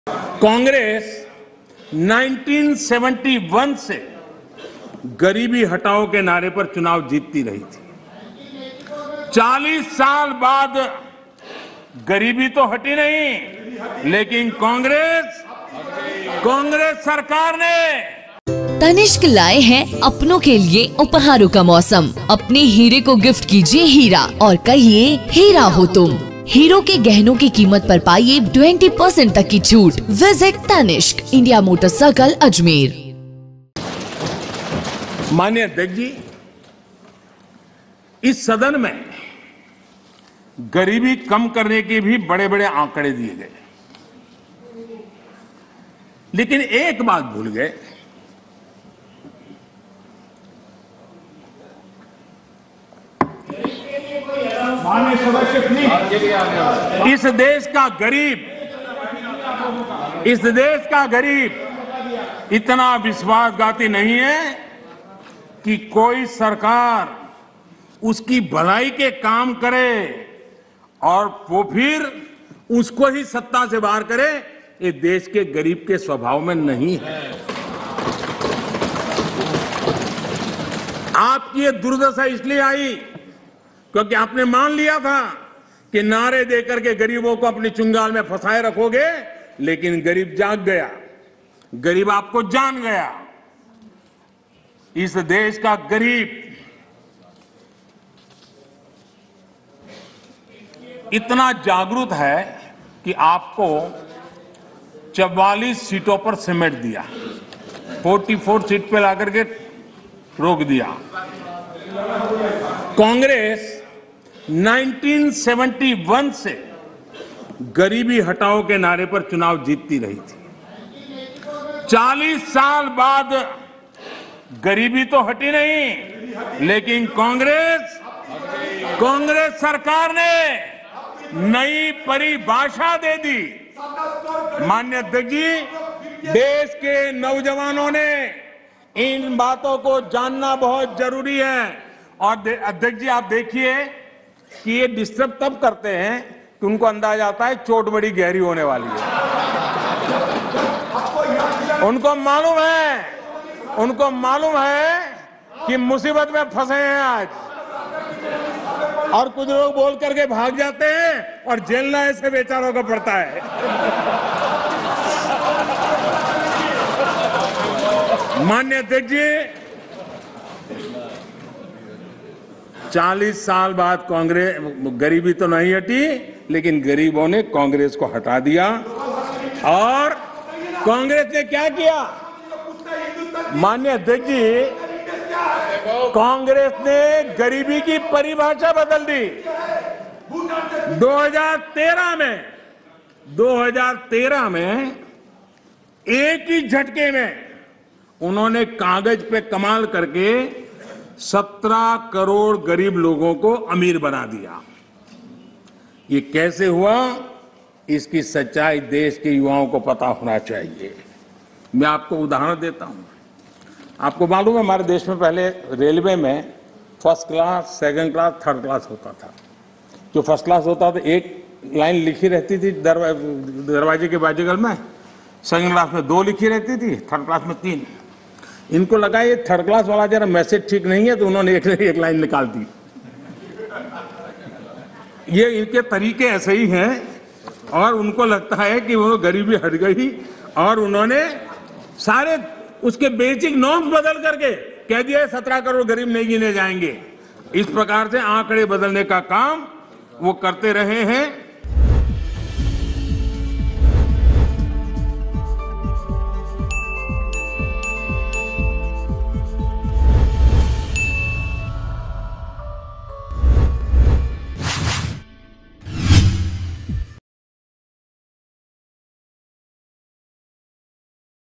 40साल तक कांग्रेस ने 'गरीबी हटाओ' के नारे पर चुनावलड़ा लेकिन गरीबीखत्म नहीं कर पाई: पीएम| MTTV INDIA
40 साल तक कांग्रेस ने 'गरीबी हटाओ' के नारे पर चुनाव लड़ा लेकिन गरीबी खत्म नहीं कर पाई: पीएम